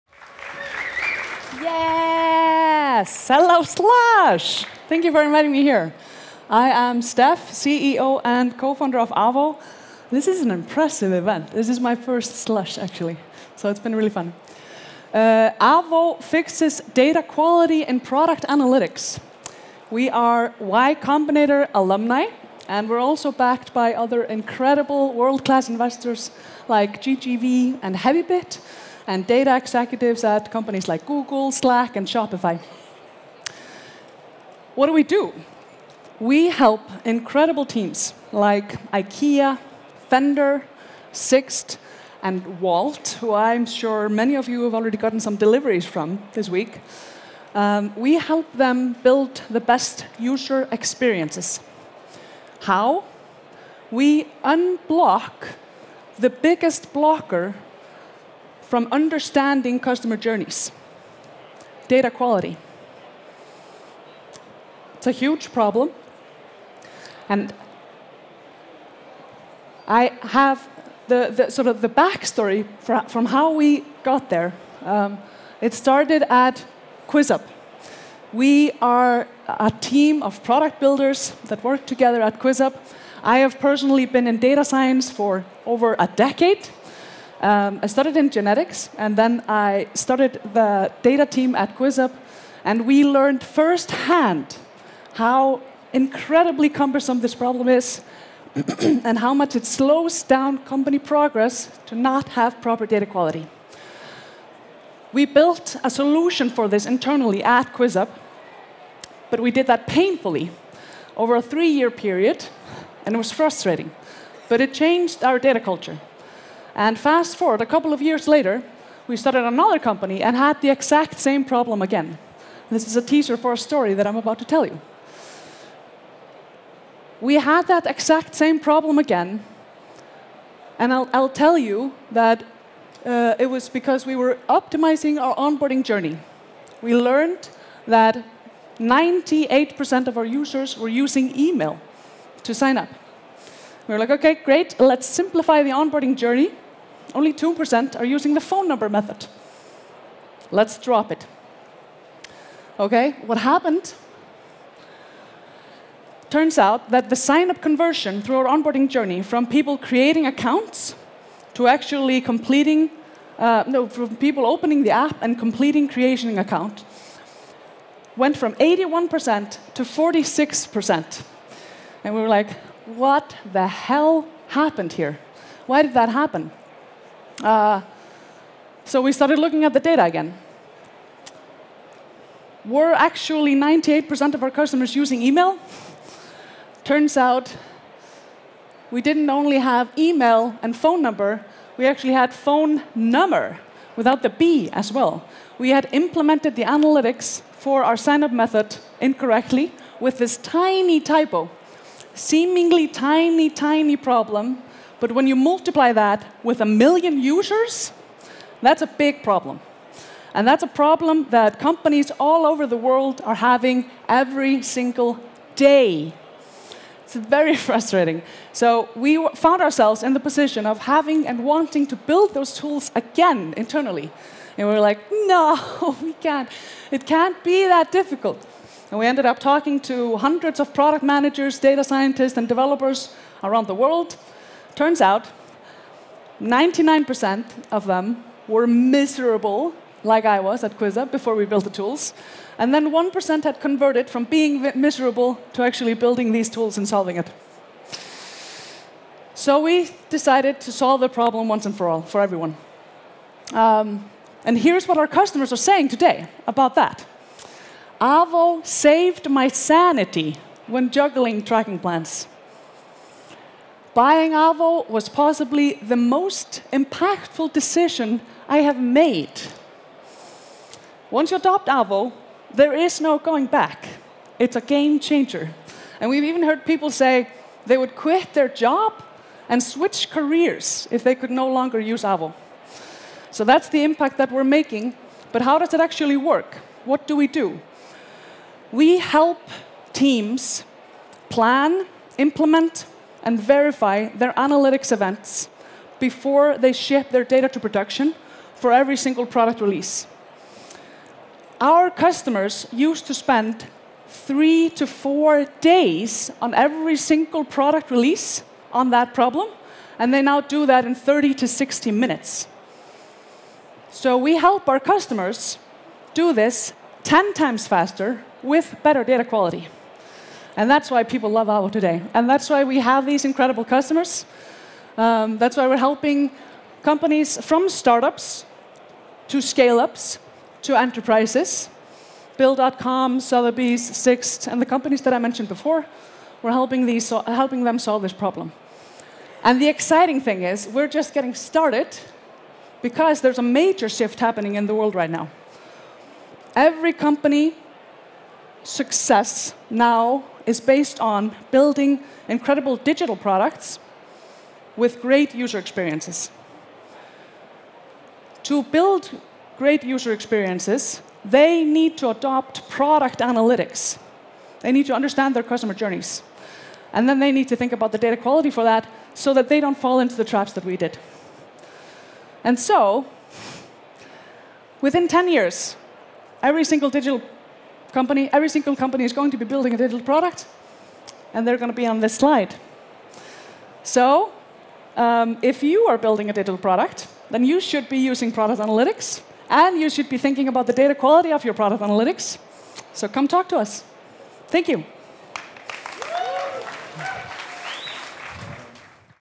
Slush Nordic Showcase: Avo (CEO & Co-Founder Stefania Olafsdóttir)
The North is known for consecutively producing high-growth companies. Many of these startups – like Kahoot, Bolt, and Veriff – were part of Nordic Showcase at the beginning of their journey. Now it’s your time to discover the 2022 twelve most promising startups from Finland, Sweden, Norway, Denmark, Iceland & Estonia.

Avo is an Icelandic SaaS startup 3 M Seed funding. Avo is the leading data quality platform for product analytics.